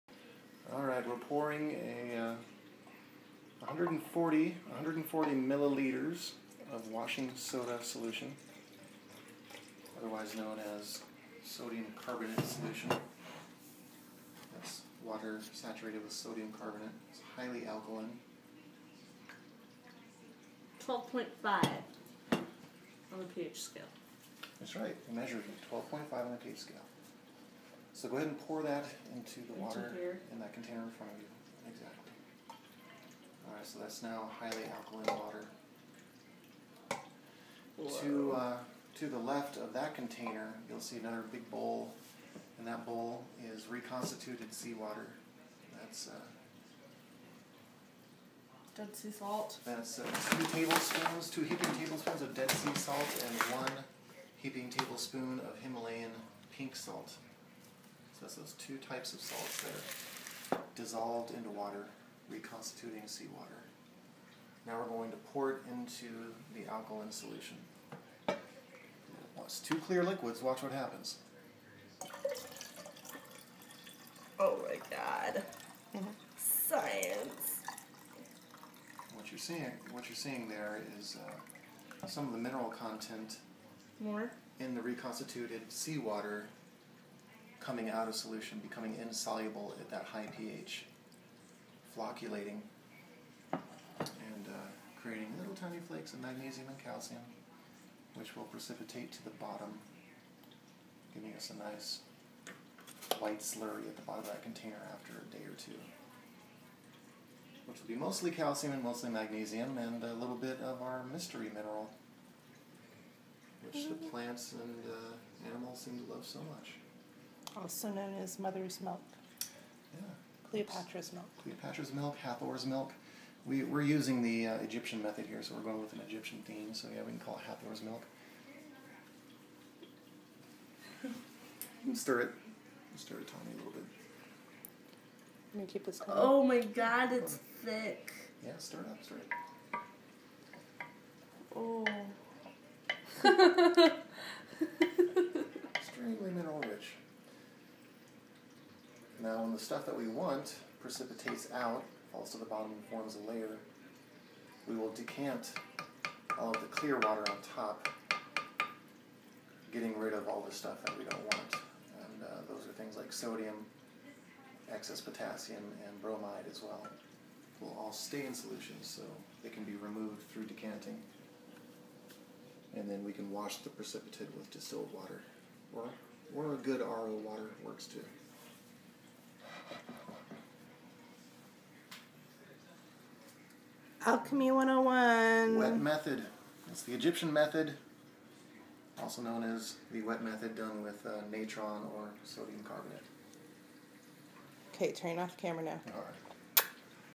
Egyptian style wet method with bokek dead sea salt (ormus)
Tani and Matthew performing part of the process to extract Ormus from Dead Sea salt , at the Pulphouse , in Arizona Featured in this video is the Egyptian Wet Method process and it is a solid reliable process We have performed The Egyptian Method many times here with consistent results We were unable to edit video at the time so we didn't show the process of making the natron solution (which takes place before what you see in the video) , and the washing process (which takes place after what you see in the video) ... Bromine, Sodium and Potassium are all removed from the precipitate by the washing process, and it is performed as follows: after settling: wash it thoroughly at least three times by decanting,mixing with pure water, letting it settle and decanting again , rinse and repeat , quite literally! (hehe) There are no Gilcrest elements in Dead Sea Salt so overshooting a PH of 10.78 by a bit is really nothing to be concerned about, therefore no need to use a PH meter if you use these measurements (as used in the video).
So ok, for the sake of detail and clarity --- STARTING FROM THE BEGINNING: First make your sodium carbonate solution by thoroughly baking 1 pound of Sodium Bicarbonate (Baking soda) in your oven on high for at least a few hours which turns the powder into Sodium carbonate (aka "Natron" or "Washing Soda") and then dissolve that natron powder into 36 ounces of water , and VOILA this is your concentrated sodium solution which will act as your alkaline reagent. It only requires about 140 milliliters of this alkaline sodium solution per batch so 36 ounces should be enough for several of these small batches. Add your 140 milliliters of sodium (natron) solution to 24 ounces of water and set it aside for later... Next make your reconstituted Dead Sea water by dissolving 2 to 3 tablespoons (yes this recipe is just THAT forgiving! =) ) into 24 ounces of water (reverse osmosis, distilled , or spring water etc) and the video demonstrates how to mix your two solutions and what to expect. Set this milky looking solution (Hathor's milk) into a dark space (like a closet) with no strong magnetic fields nearby ...You can wrap the container with aluminum foil for further protection from RF or other high frequency EM fields while the precip is settling, but honestly Ormus is very stable with this much sodium present, but that will change after the washing is finished. Potency degrades quickly after the washings, so you want to use it over the next few days after the final wash This decanting/washing process can begin after the precip is fully settled which may take a few days , but it will settle faster after the first wash Perhaps i will post an edited video showing all of the steps in more detail in the near future NOTE: You can dissolve the finished precip in fresh Lemon juice, I highly recommend mixing this with something that contains live beneficial bacteria (like a good Yogurt , Kefir , Kombucha, Kevita, or EM1 if you are feeding plants), this will enhance the effects of the Ormus.